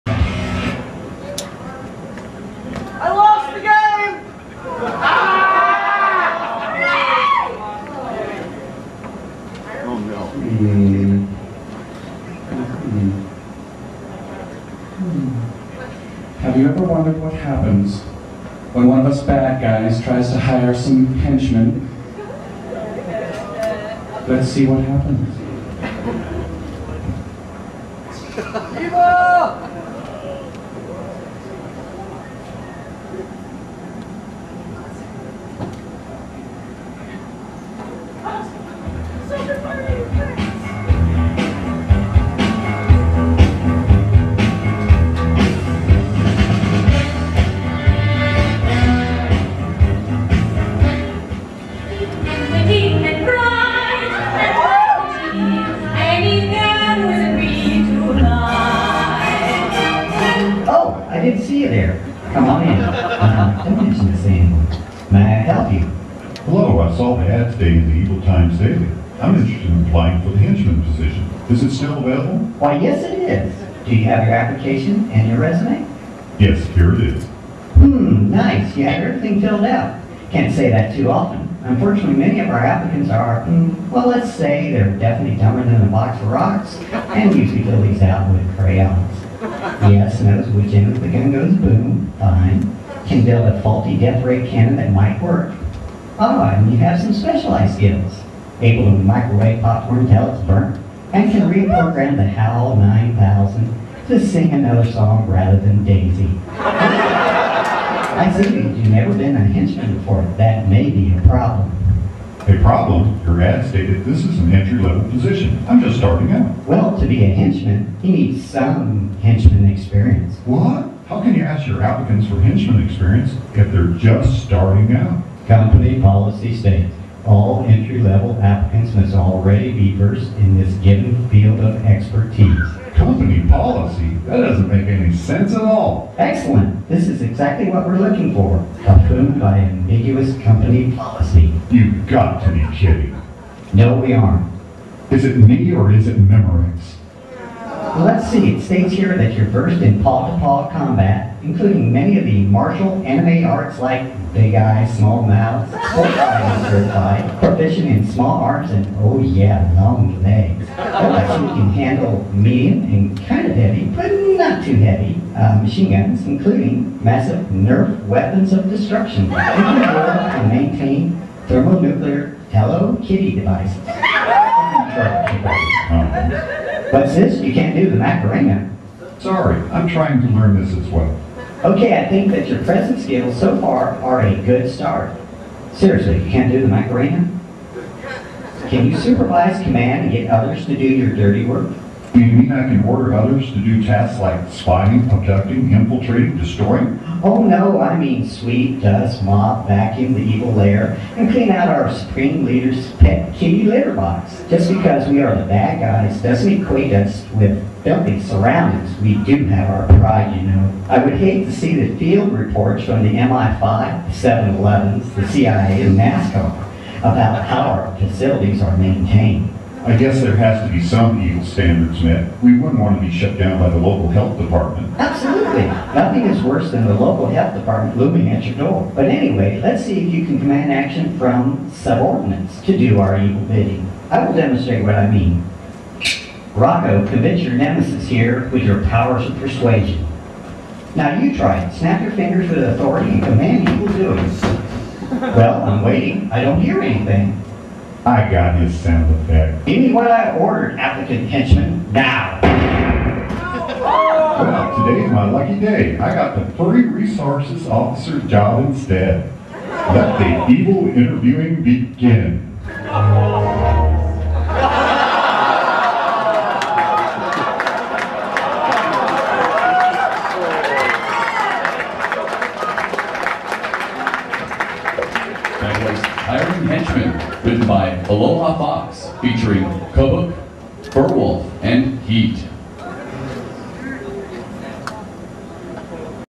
Hiring Henchmen
Hiring Henchmen skit from Furry Night Live, Further Confusion 2007.